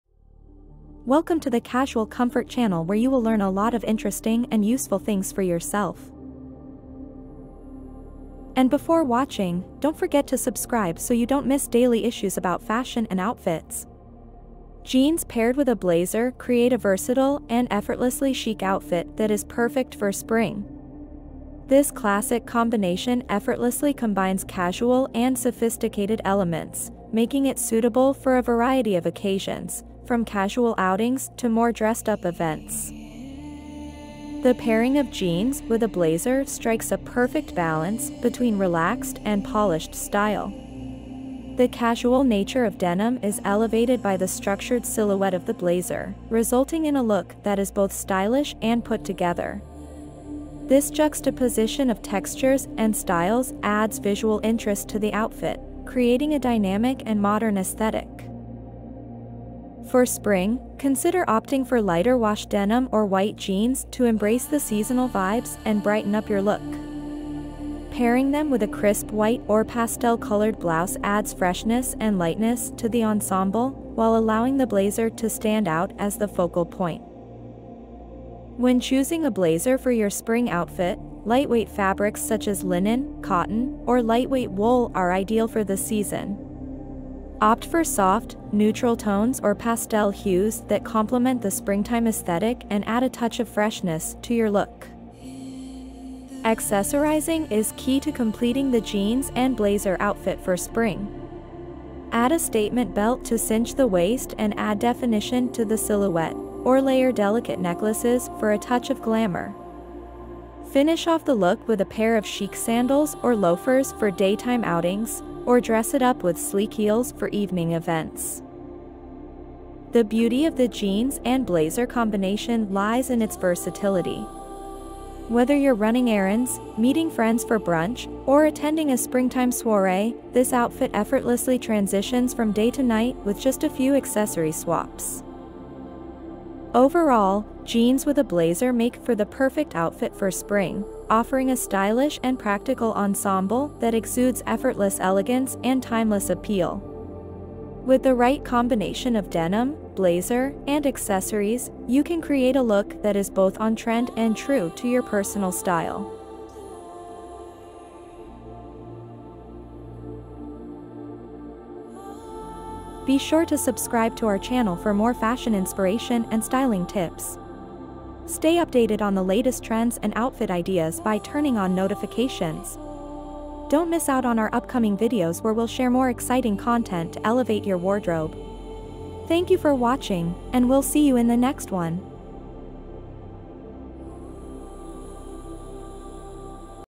Jeans & Blazer | Chic Outfit Ideas 2024: How to Style Jeans with a Blazer for Spring
Elevate your spring wardrobe with chic outfit ideas for women in 2024! In this video, we showcase the timeless combination of jeans with a blazer, offering versatile and stylish looks for any occasion. From casual weekend outings to polished office attire, learn how to effortlessly pair your favorite jeans with a sophisticated blazer to achieve a chic and polished ensemble. Our fashion tips and styling tricks will help you master the art of balancing comfort and elegance while staying on-trend this season. Discover the perfect outfit formula to elevate your spring wardrobe and make a statement wherever you go!